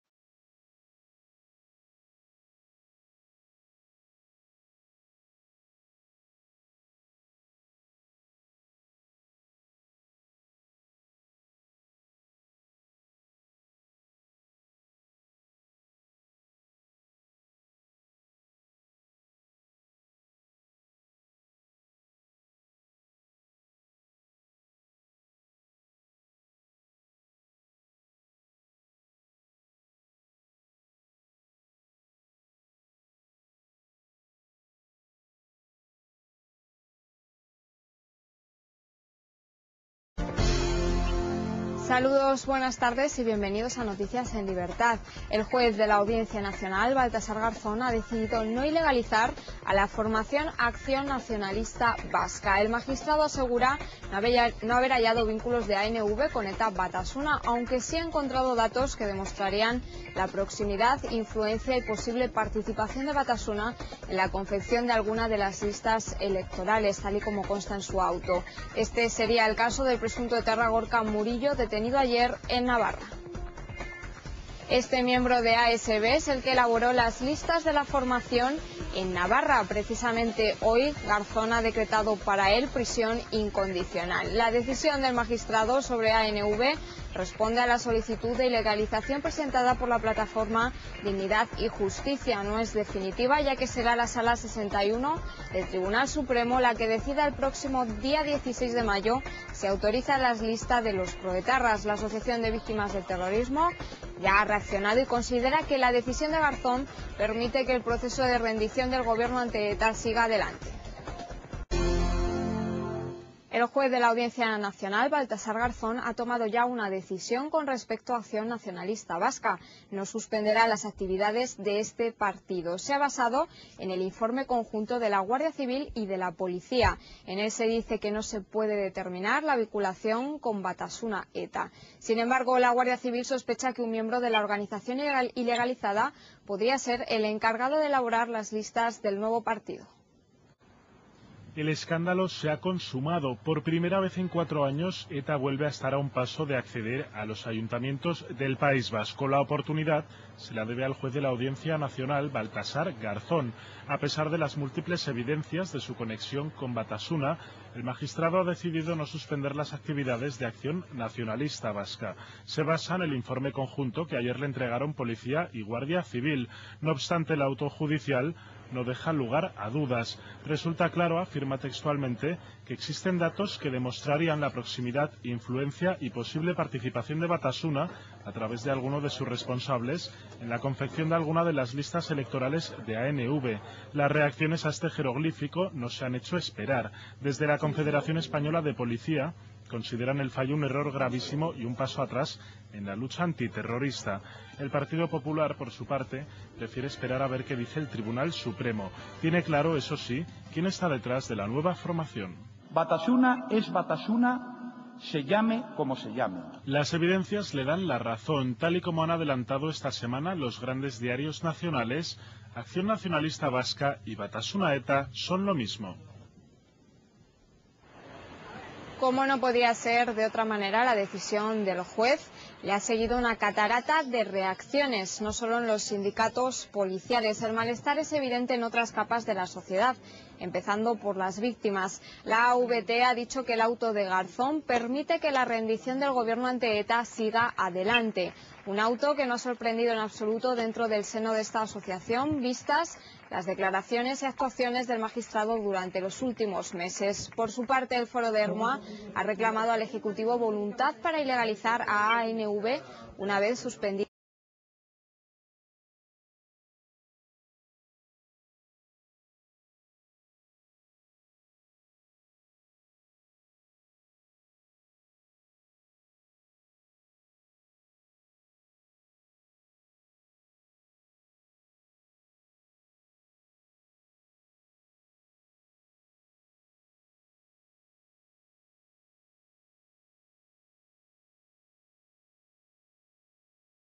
Garzón satisface la segunda condición de ETA,convalida ANV
Garzón satisface la segunda condición de ETA y convalida sus listas en pleno puente festivo
 Tras estudiar el informe unificado de las fuerzas de Seguridad que exigió con un plazo de tan sólo 48 horas, Baltasar Garzón ha sentenciado que no hay pruebas de la vinculación de ANV con Batasuna-ETA. Al magistrado no le han bastado las evidencias de su relación con la banda. Tampoco al Gobierno, que queda reforzado en su decisión de no instar a la ilegalización del partido. El hecho de que Garzón haya dictado el auto, que incluye una orden de prisión contra Gorka Murillo, en pleno puente, no ha evitado las reacciones. Mariano Rajoy ha recordado ahora al Gobierno que su deber es que Batasuna no esté en los comicios amparada por estas u otras siglas.El Gobierno ya había suministrado un informe policial ad hoc y la vicepresidenta se había lavado las manos, remitiéndose a lo que decidiera Garzón.

El juez instructor ha resuelto no prohibir las listas de ANV, tal y como estaba previsto, a la vista del informe conjunto que recibió este viernes de la Guardia Civil y de la Policía sobre ANV, en el que se señalaba que no se puede establecer que ANV fuera un instrumento de ETA-Batasuna, aunque sí podría haber indicios de una utilización de este partido por parte de Batasuna.

Baltasar Garzón solicitó los informes policiales después de que la asociación de víctimas Dignidad y Justicia, le reclamara dos veces la suspensión de ANV por entender que forma parte del "núcleo Batasuna, ya suspendido".